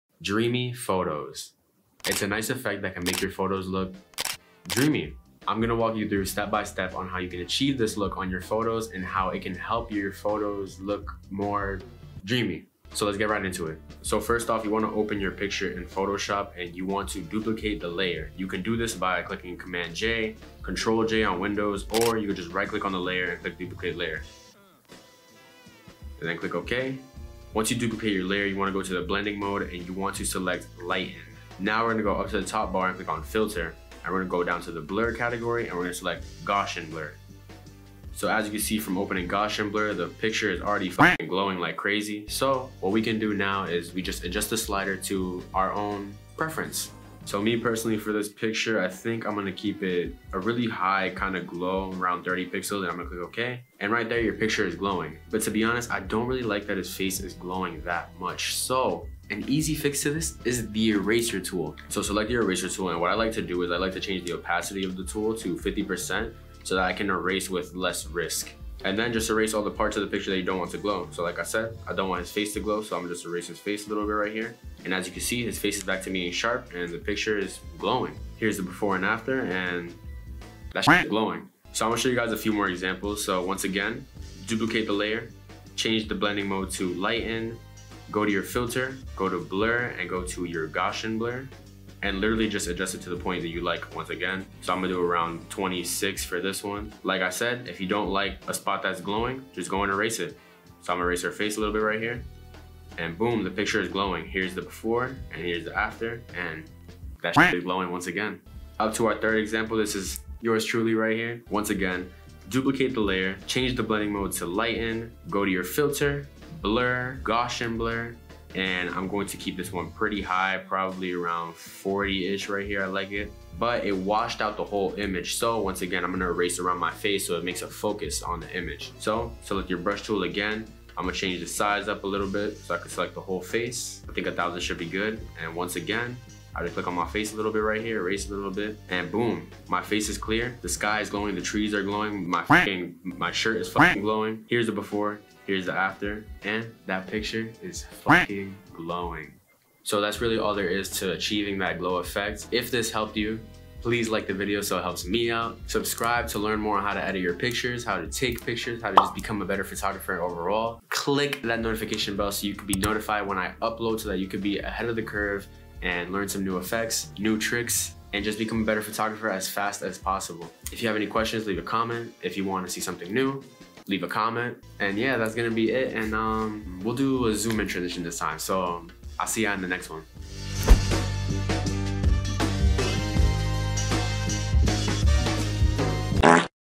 DREAMY GLOW EFFECT - Photoshop Tutorial
here in this video, i teach you how to make your pictures have a soft glow effect. this effect will make any of your photos look dreamy! i use photoshop in this tutorial to teach yall :D

if you liked the video then you should tickle that like button for me real quick

thank you for checking down here :)